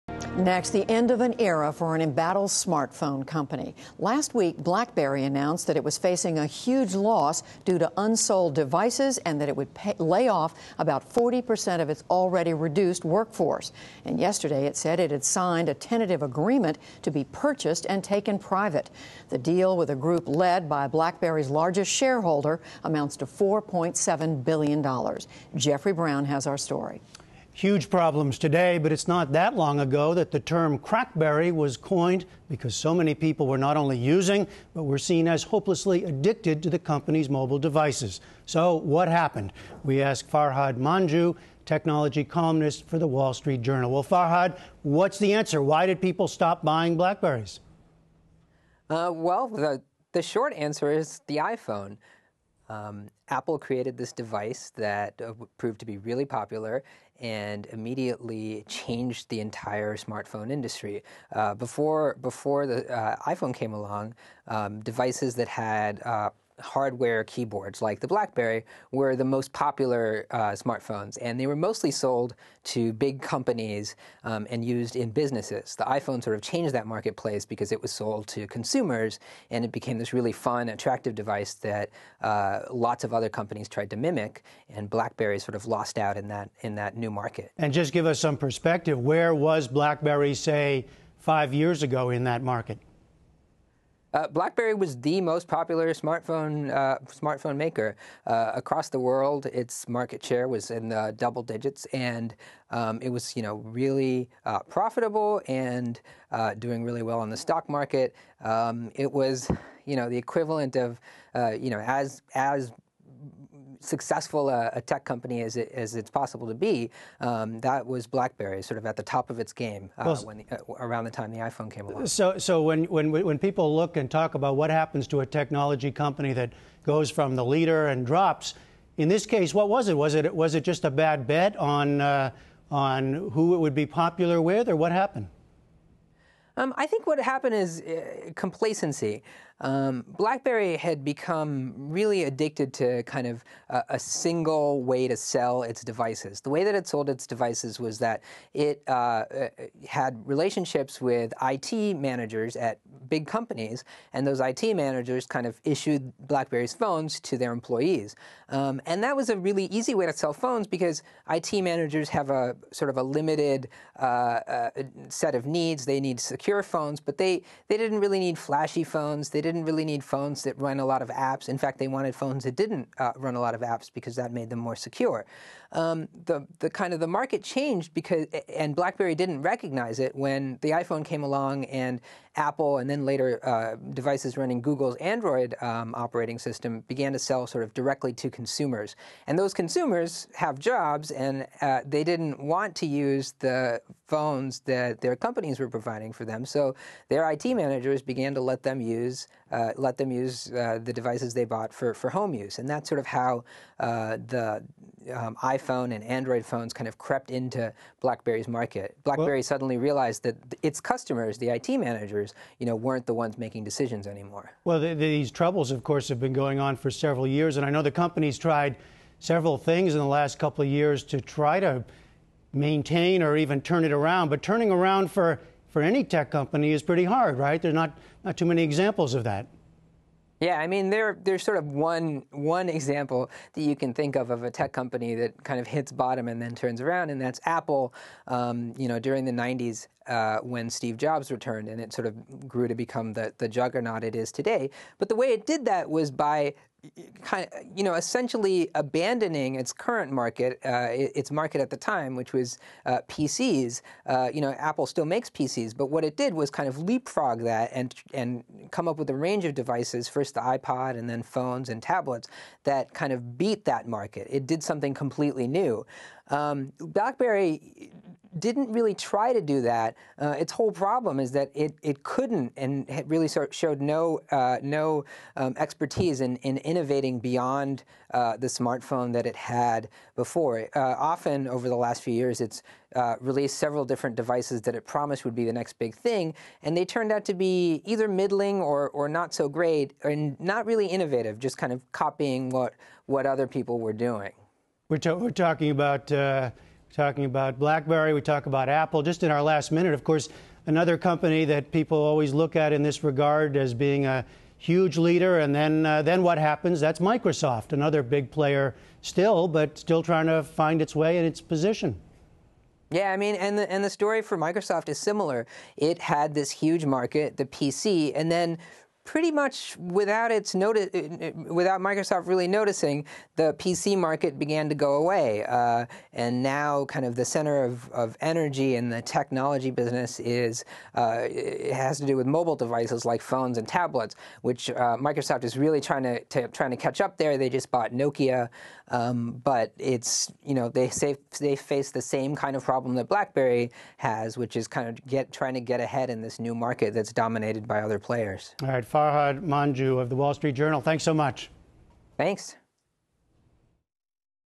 How Did Blackberry Fall Behind Other Smart Phones?
At the top of its game, Blackberry was the world's most popular mobile device, but now the struggling smart phone maker has announced a $4.7 million buyout. What mistakes did Blackberry make that its competitors got right? For insight, Jeffrey Brown talks to Farhad Manjoo of The Wall Street Journal.